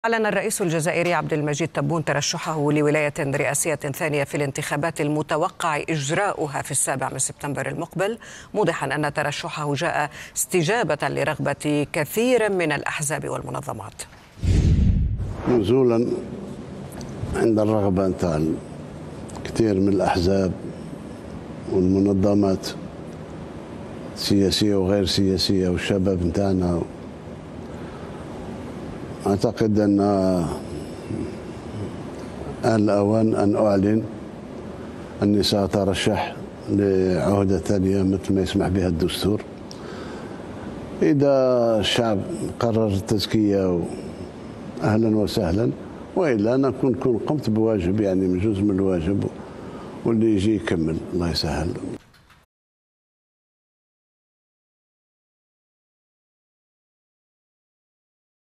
الرئيس الجزائري يعلن ترشحه لولاية رئاسية ثانية
الرئيس الجزائري عبدالمجيد_تبون يعلن ترشحه لولاية رئاسية ثانية

لمعرفة أحدث المستجدات.. عبر البث المباشر من هنا
وبإمكانكم متابعة الحدث على منصات التواصل